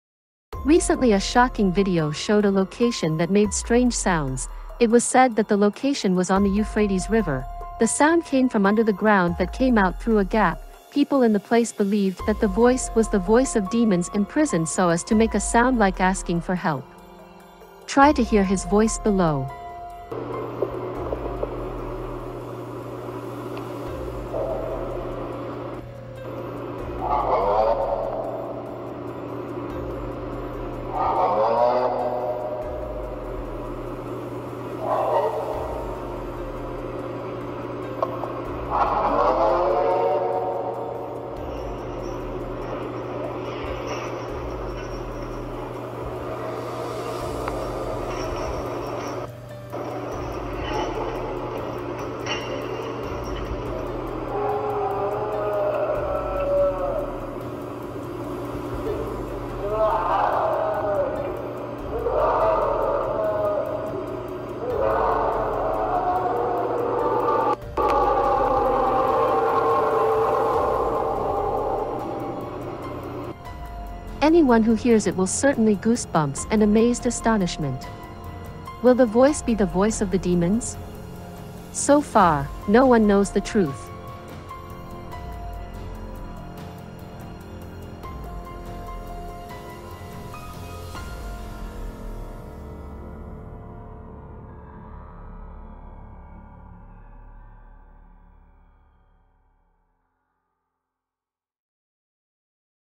Surprisingly !! The voice of the demons under the Euphrates was caught on camera
Recently a shocking video showed a location that made strange sounds, it was said that the location was on the Euphrates river., the sound came from under the ground that came out through a gap, people in the place believed that the voice was the voice of demons imprisoned so as to make a sound like asking for help.